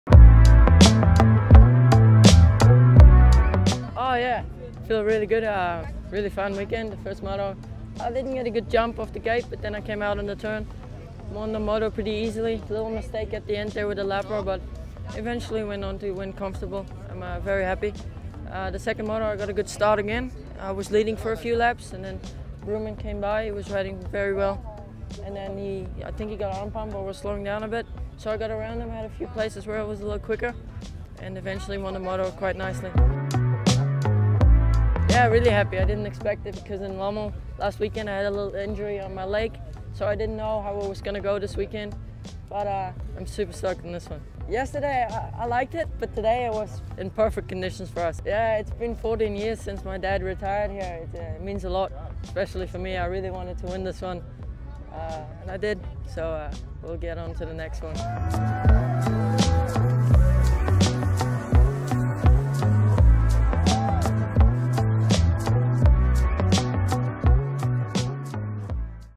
Everts makes history: Liam Everts pays tribute to his father Stefan after Matterley double win
On the same track where his dad Stefan ended his career with a stunning double win in the Motocross of Nations in 2006, Liam Everts took a fine double win in the EMX125 opener at the British GP at Matterley.
And he even made the pass for the lead in the same corner where Stefan overtook James Stewart to take the Nations win 14 years ago. And like Stefan, he did it on the pegs around the outside!
It was the first time Liam topped the podium at a GP, at the same track his father Stefan last stood on top of the podium. And Liam's grandad Harry, a four-times world champ, was also on hand to witness history in the making.
15-year-old Liam talks of his win and pays tribute to his father Stefan who came close to losing his life last year after battling Malaria.